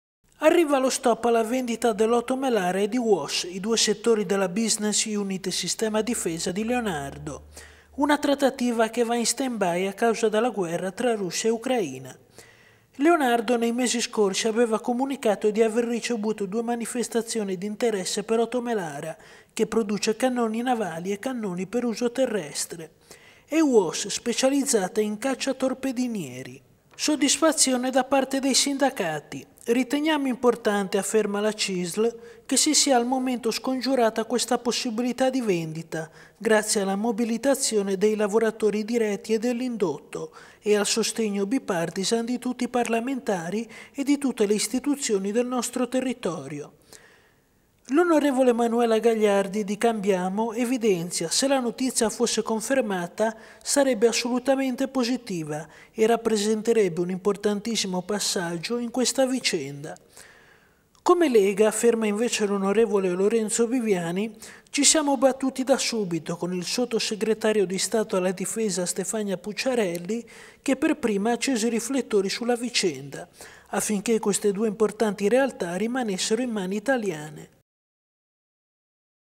Leonardo - Oto Melara, stop alla vendita 10-03-2022
Arriva lo stop alla vendita dell’ex Oto Melara, a riportarlo alcune agenzie di stampa. Soddisfazione dei politici spezzini.